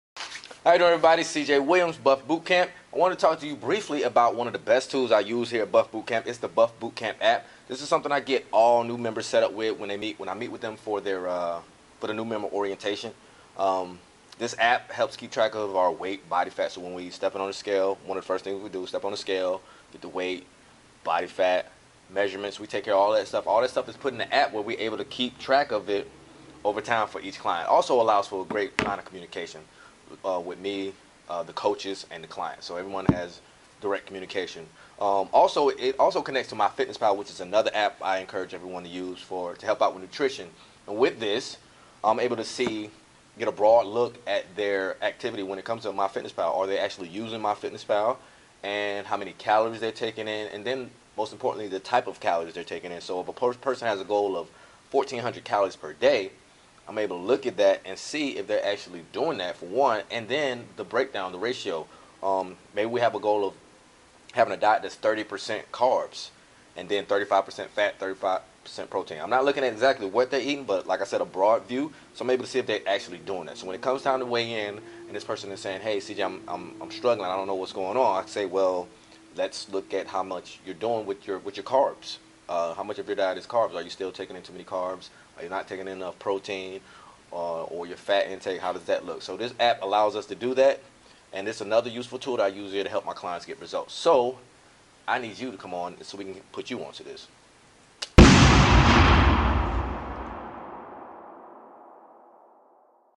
The BUF Boot Camp App Does It All
It offers a mode of communication between members and trainers.
It allows both members and trainers access to member progress
weight
body fat
measurements
progress pics